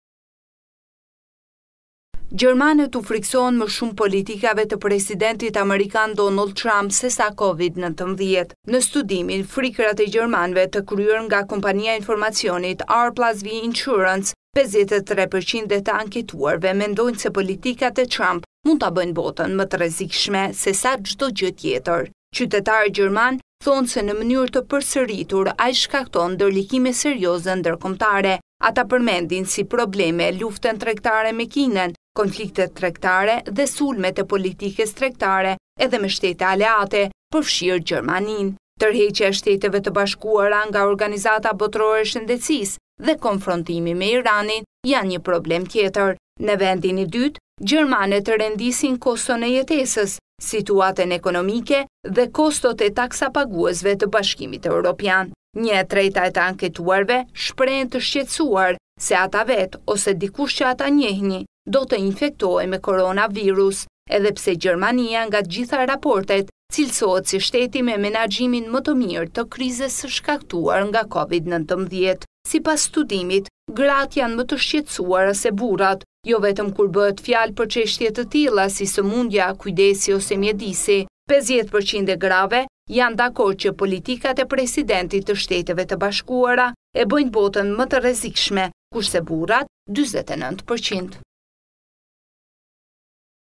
Gjermanët të frikësuar më shumë nga politikat e Presidentit Trump se nga Covid-19
Gjermanët u friksohen më shumë politikave të presidentit amerikan, Donald Trump sesa COVID-19.

Në studimin “Frikërat e Gjermanëve” kryer nga  ‘R+V Insurance’,  53% e të anketuarve mendojnë se politikat e Trump mund ta bëjnë botën më të rrezikshme, sesa çdo gjë tjetër.

Qytetarët gjermanë thonë se në mënyrë të përsëritur ai shkakton ndërlikime serioze ndërkombëtare.

Ata përmendin si probleme, luftën tregtare me Kinën,  konfliktet tregtare dhe sulmet e politikës tregtare edhe me shtete aleate, përfshirë Gjermaninë.

Tërheqja e SHBA nga OBSH  dhe konfrontimi me Iranin.

Në vendin e dytë dhe të tretë, gjermanët rendisin koston e jetesës, situatën ekonomike dhe kostot e taksapaguesve të BE-së  për shkak të krizës së shkaktuar nga Covid-19.

Një e treta e të anketuarve u shprehën se ata vetë  ose dikush që ata e njihnin do të infektohej me koronavirus edhe pse Gjermania nga të gjitha raportet cilësohet shteti me menaxhimin më të mirë të krizës së shkaktuar nga Covid-19.

Sipas studimit gratë janë më të shqetësuara se burrat - jo vetëm kur bëhet fjalë për çështje të tilla si sëmundja, kujdesi ose mjedisi.

57 përqind e grave janë dakord që politikat e Presidentit të SHBA e bëjnë botën më të rrezikshme, kurse burrat përfaqësohen me 49 përqind.